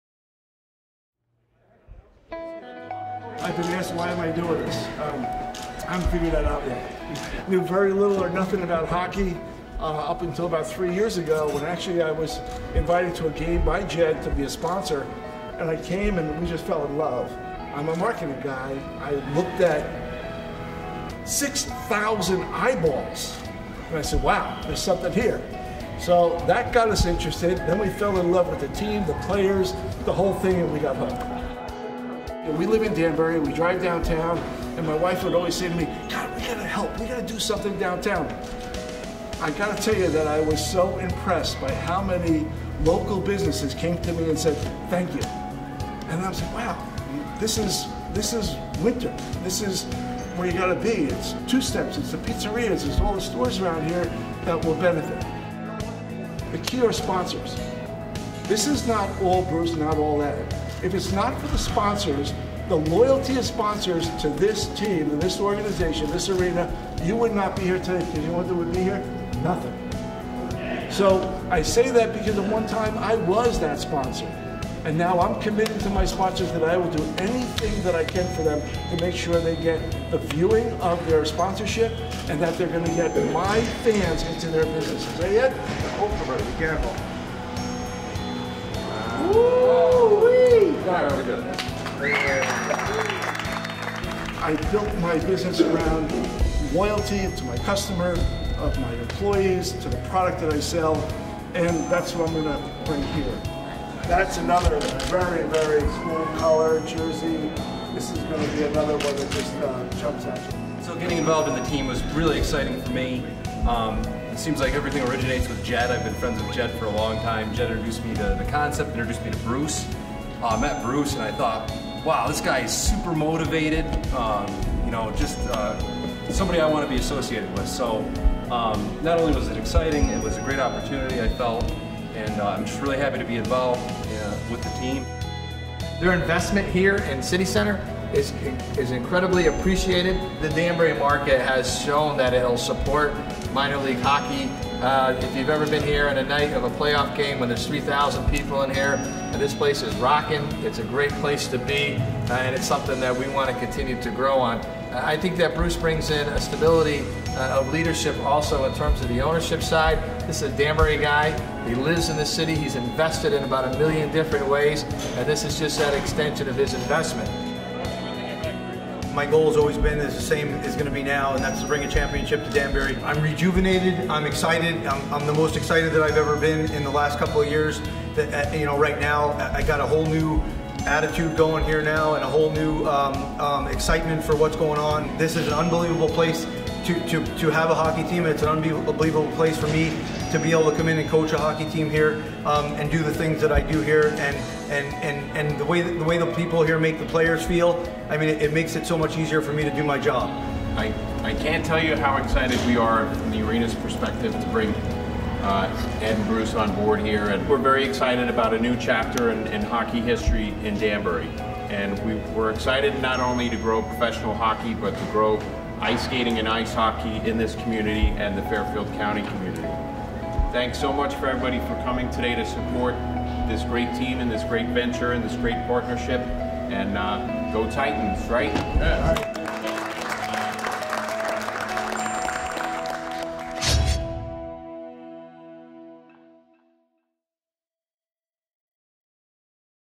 Introducing the Danbury Titans to the City of Danbury
Introduction video for the Danbury Titans of the Federal Hockey League.